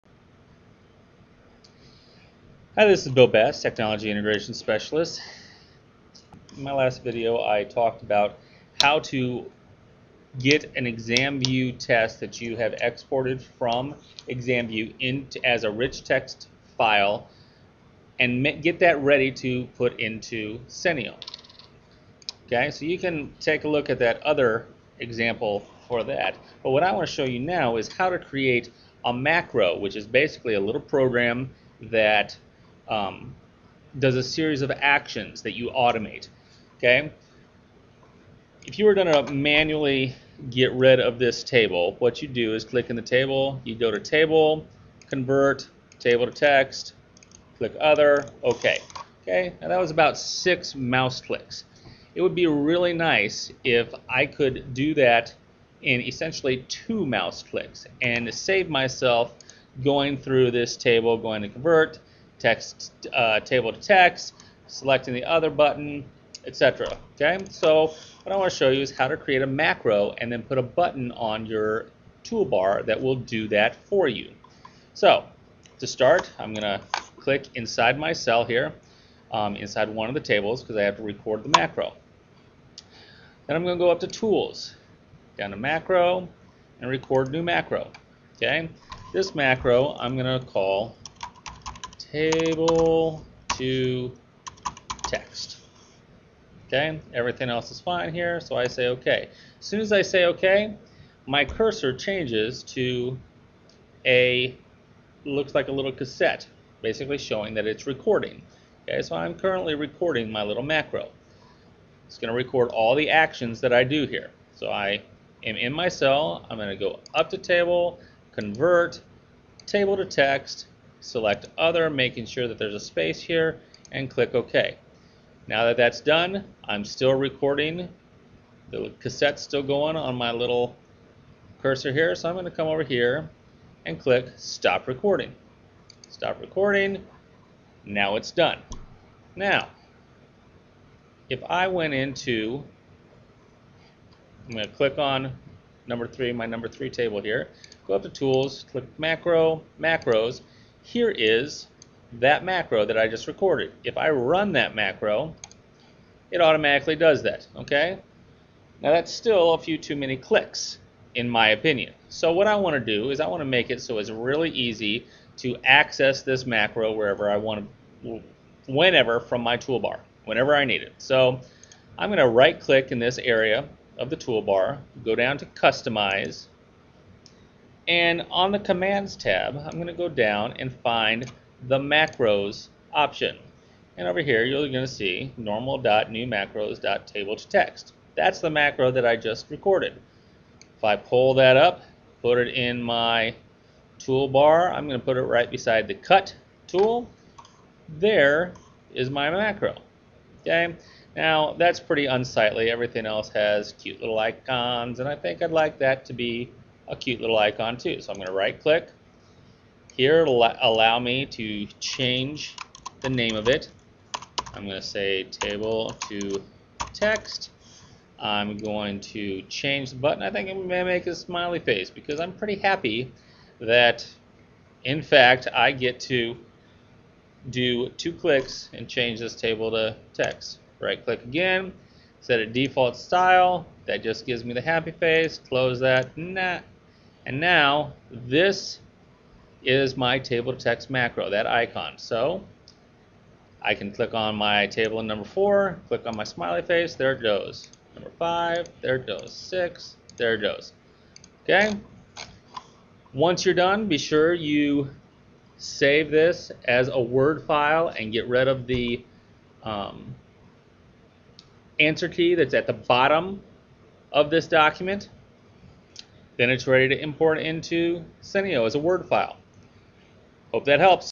Creating a Macro in MS Word 2003
This video was created in 2009 describing how to create a macro in Word 2003 for the purpose of using ExamView software with the SMART Senteo system in classrooms.